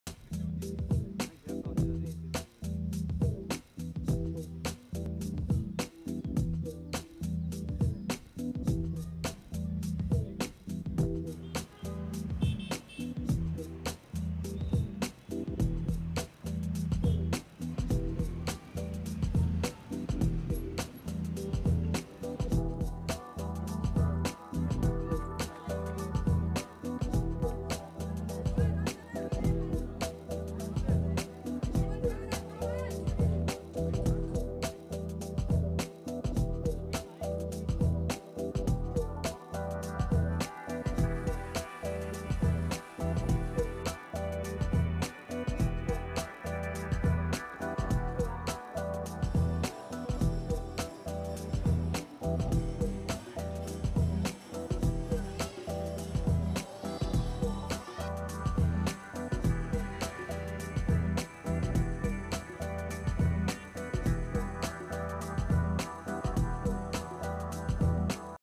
Indians top list of foreign tourists visiting Nepal | OneIndia News
As many as 2,60,124 Indian tourists have visited Nepal in the last ten months, taking the number of tourists visiting the neighbouring country to a whopping one million.